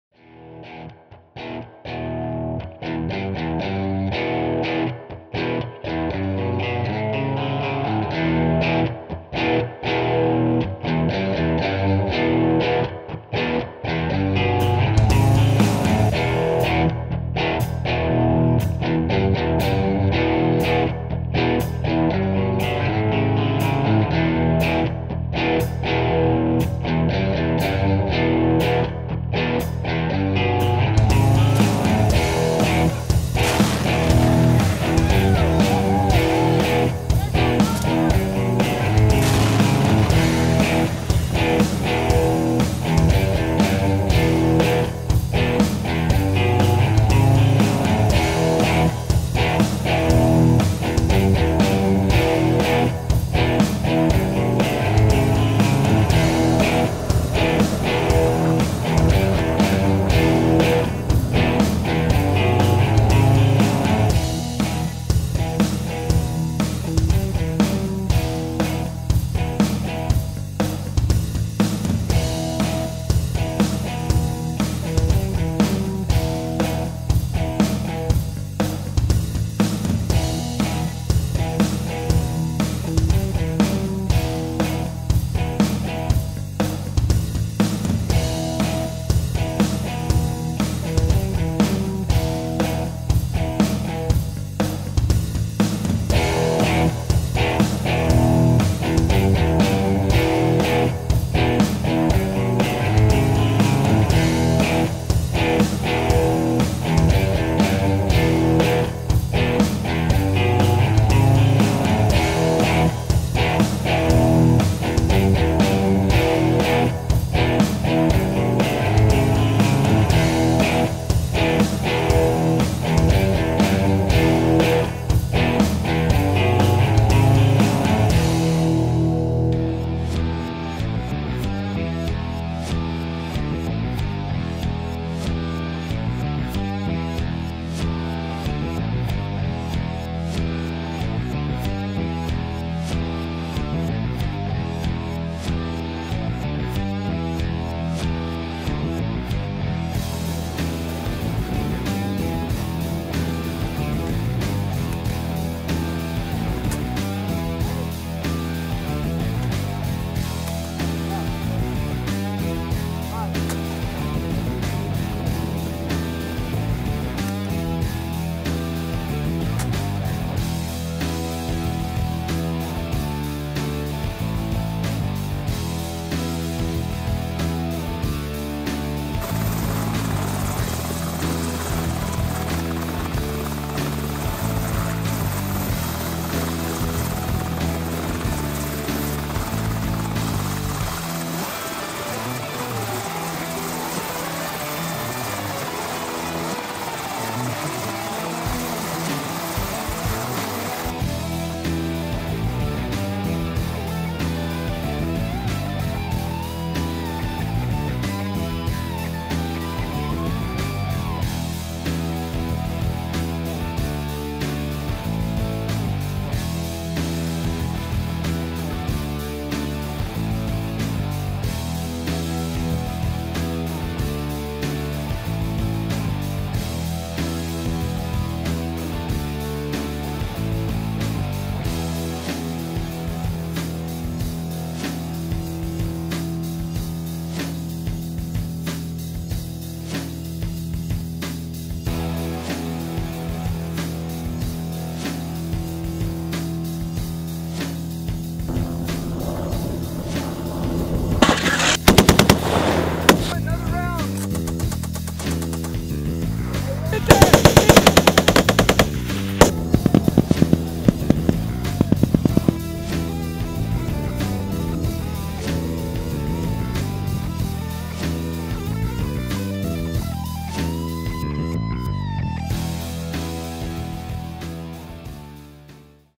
1st Brigade In Action
Video and photo compilation highlighting the brigade throughout the years.